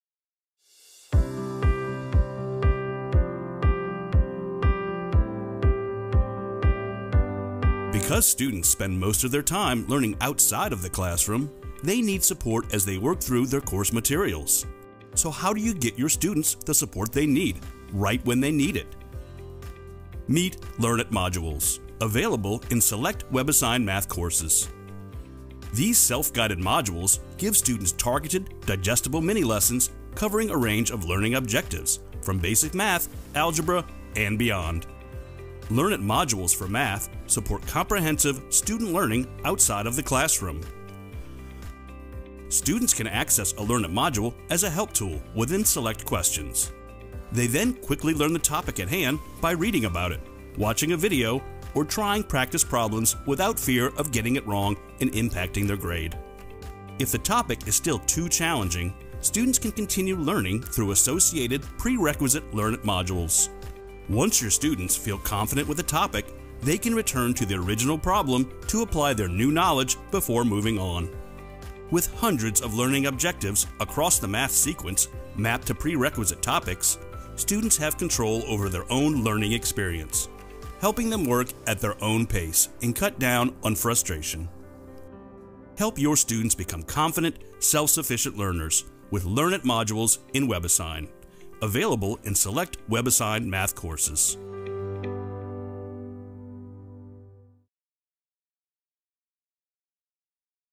Build Confident Learners with Learn It Modules
Help your students become confident, self-sufficient learners with new Learn It modules. Learn It modules address your students’ knowledge gaps with just-in-time instruction that meets their diverse learning styles. Embedded within questions, students are presented a Learn It for immediate targeted instruction and practice on that topic using narrative, videos and tutorials – all in one place. If the topic is still too challenging, students can choose to continue learning through associated prerequisite Learn Its until they feel confident in their knowledge and preparedness.